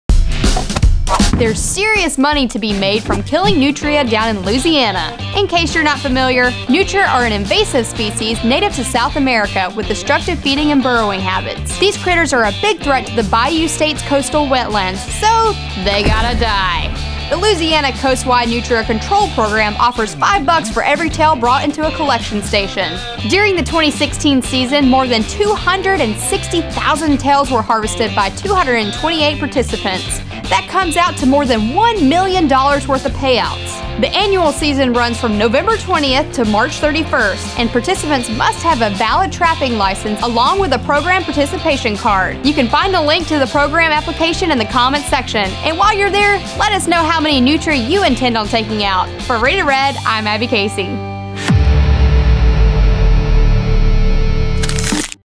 There’s Some Serious Money to Be Made from Hunting Nutria Down in Louisiana
In case you’re not familiar, nutria are an invasive species native to South America, with destructive feeding and burrowing habits.
These critters are a big threat to the Bayou State’s coastal wetlands, so they gotta die. Now's your chance!

Check this out!!!